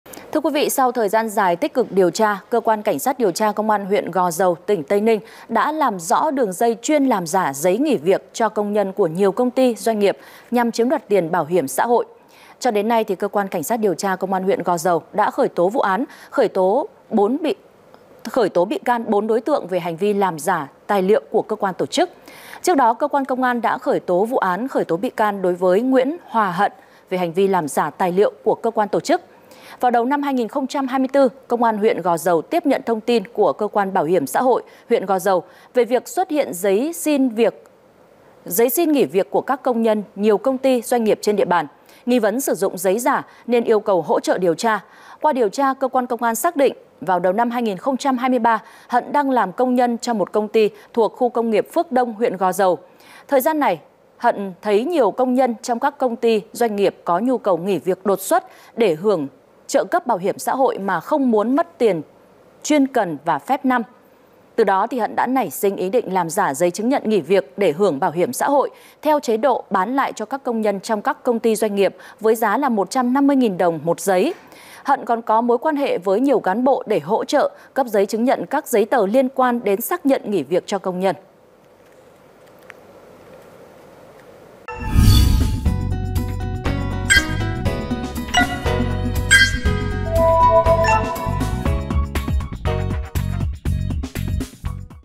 Làm giả giấy nghỉ việc cho công nhân để chiếm đoạt tiền bảo hiểm xã hội | ANTV
ANTV | Đến nay, Cơ quan CSĐT Công an huyện Gò Dầu đã khởi tố vụ án, khởi tố bị can đối với: Bùi Thị Bích Trang (SN 1987, ngụ ấp Sóc Lào, xã Đôn Thuận, thị xã Trảng Bàng), Huỳnh Khải An (SN 1999, ngụ ấp Hiệp Bình, xã Hoà Thạnh, huyện Châu Thành), Lê Thị Tố Quyên (SN 1998, ngụ ấp Xóm Bố, xã Hiệp Thạnh, huyện Gò Dầu) và Phạm Thị Hạnh (SN 1991, ngụ ấp Lộc Tân, xã Lộc Ninh, huyện Dương Minh Châu) về hành vi làm giả tài liệu của cơ quan, tổ chức; sử dụng tài liệu giả của cơ quan tổ chức.

Mời quý vị khán giả xem thêm: Tin tức an ninh trật tự nóng, thời sự Việt Nam mới nhất 24h trưa ngày 27/9